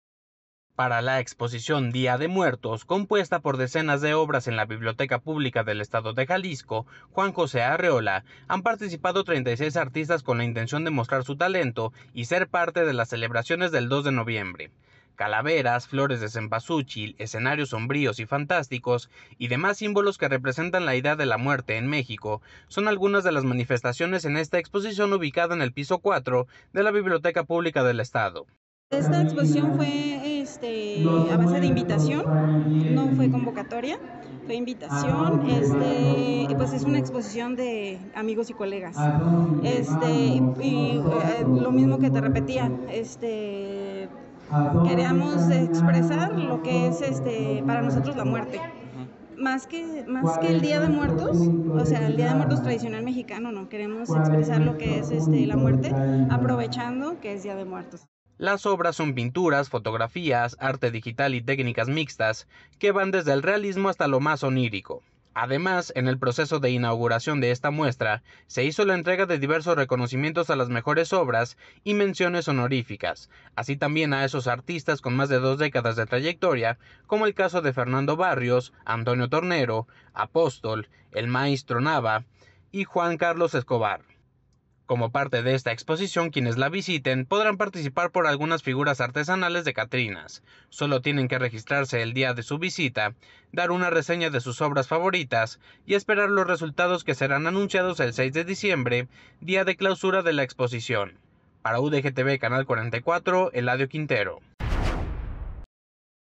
Artistas plásticos exponen sus obras con motivo del Día de Muertos en Biblioteca Pública del Estado
Para la exposición 'Día de Muertos', compuesta por decenas de obras en la Biblioteca Pública del Estado de Jalisco "Juan José Arreola" (BPEJ) han participado 36 artistas con la intención de mostrar su talento y ser parte de las celebraciones del 2 de noviembre.